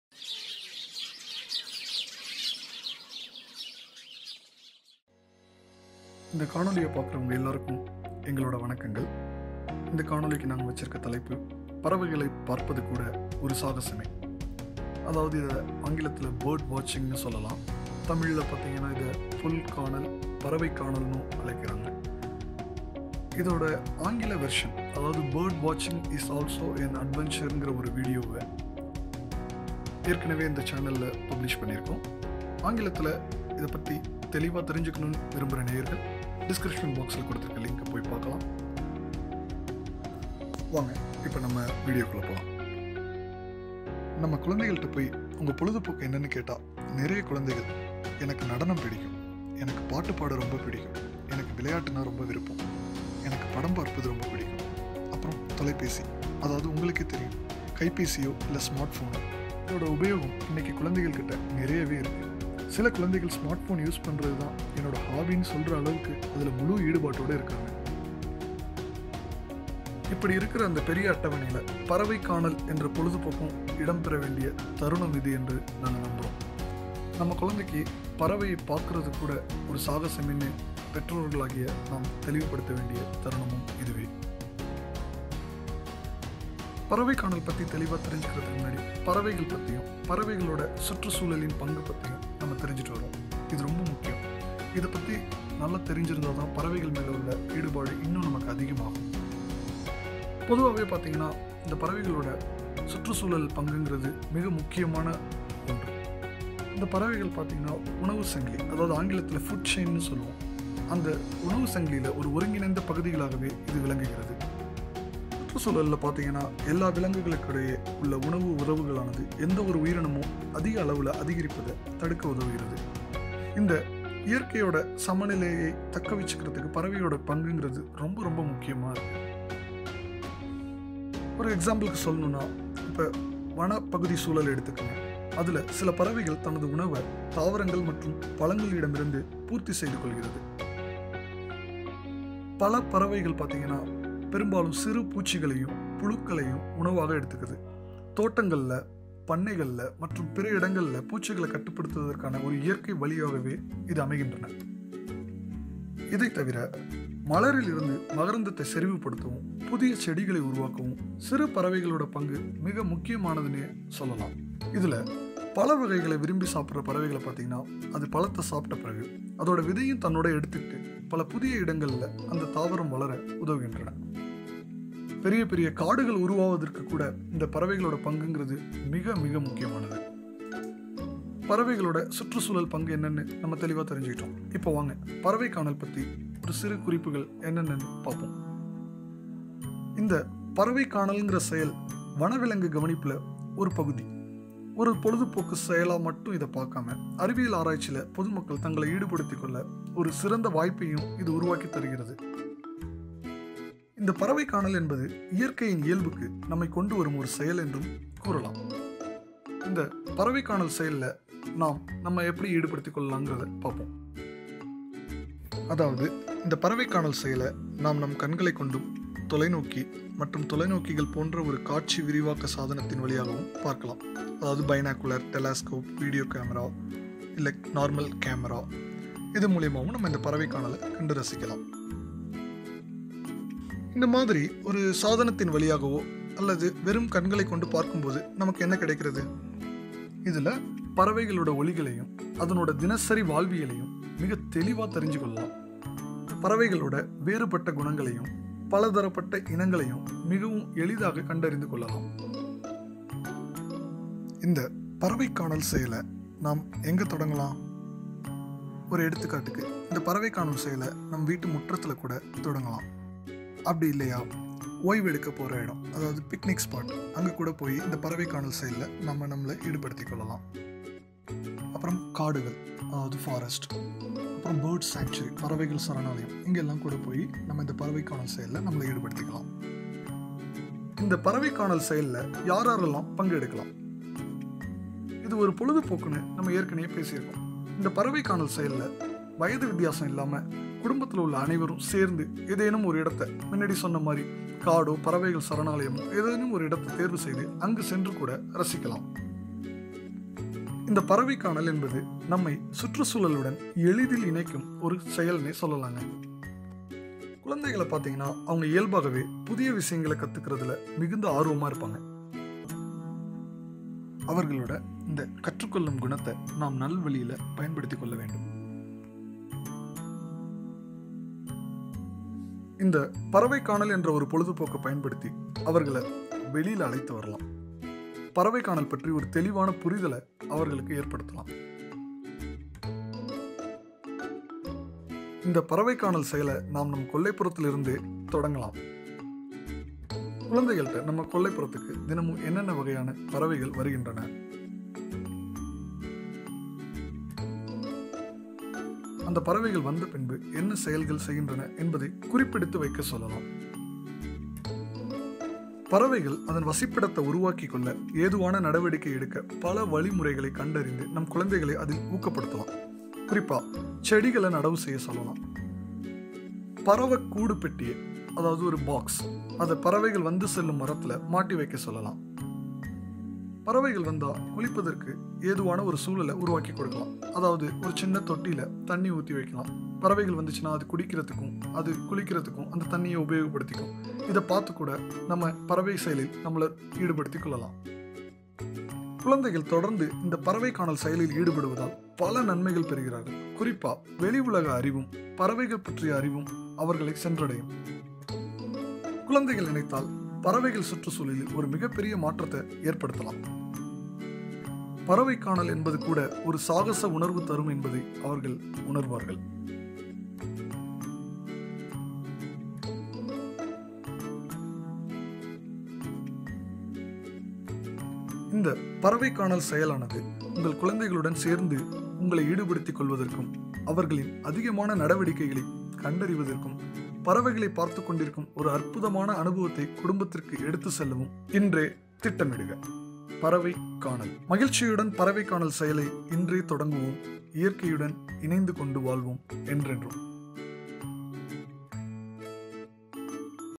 "பறவை காணல்" அல்லது "புள் காணல்" - பறவைகளை பார்ப்பதுகூட ஒரு சாகசமே
உங்களுடைய பொழுதுபோக்கு என்னவென்று கேட்டால் அதில் பெரும்பாலானோர் சொல்வது ஆடல், பாடல், விளையாட்டு, நண்பர்களுடன் நேரம் செலவழிப்பது, திரைப்படம் பார்ப்பது, கைபேசி உபயோகிப்பது என்று அடிக்கிக்கொண்டே போகலாம். அப்படி உள்ள இந்த பெரிய அட்டவனையில் “பறவை காணல் (அ) புள் காணல்” என்ற பொழுதுபோக்கும் இடம் பெற வேண்டும் என்ற ஒரு ஆவலுடன் உருவாக்கியதே இந்த காணொலி.

உங்கள் குழந்தைகளுடன் சேர்ந்து ஈடுபடுவதற்கும் அவர்களின் அதிகமான நடவடிக்கைகளைக் கண்டறிவதற்கும், பறவைகளைப் பார்த்துக் கொண்டிருக்கும் ஒரு அற்புதமான அனுபவத்தை குடும்பத்திற்கு எடுத்துச்செல்லவும் இன்று திட்டமிடுக.

நன்றி : கவிஞர் மகுடேசுவரன் அவர்களுக்கு (Bird watching என்ற ஆங்கில தலைப்பை எப்படி தமிழில் பயன்படுத்தலாம் என்று தொலைபேசியில் கேட்டபோது அதை “பறவை காணல் அல்லது புள் காணல்” என்று எழுதுமாறு பரிந்துரைத்ததற்கு).

மகிழ்ச்சியுடன் பறவைகளை கவனிப்போம், இயற்கையுடன் இணைத்துகொண்டு வாழ்வோம், என்றென்றும்.